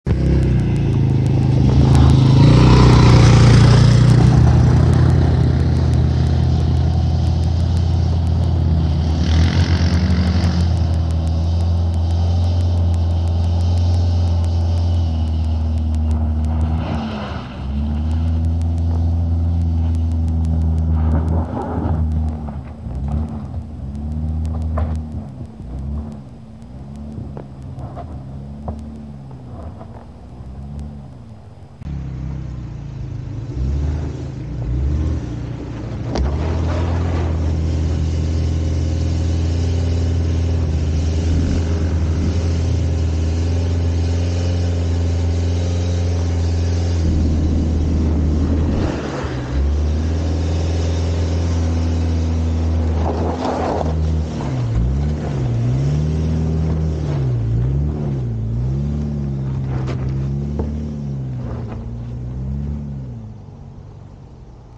Subaru XV Diagonal climb: wait for x-mode
whats the longest it took for your x-mode to figure things out? I counted at least 7 seconds for the first section. a common rookie mistake is too leave the throttle too soon but I honestly did not expect that.

odd thing is, you can see wheels breaking the entire time.. it is as if it eventually enters a hidden "lower" gear, which kind of sounds silly but thats how it felt inside.

ps. next time going to record OBD readings too so you can see the RPM and throttle in the video :)

x-mode on, tc on.

Subaru Crosstrek xv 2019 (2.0L engine)
Geolander g015 tires